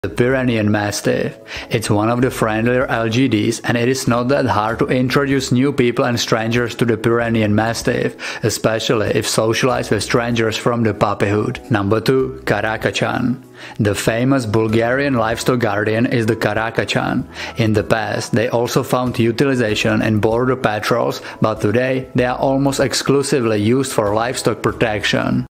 Pyrenean Mastiff and Karakachan - Perfect Livestock Guardian Dog Breeds
Livestock guardian dogs have been selectively bred for specific traits for thousands of years, which means, most of the LGDs have quite similar personality, such as incredible loyalty to its owners, but also to the livestock, very low or even no prey drive, natural protectivness and alertness, big strength, high level of independence and decisiveness.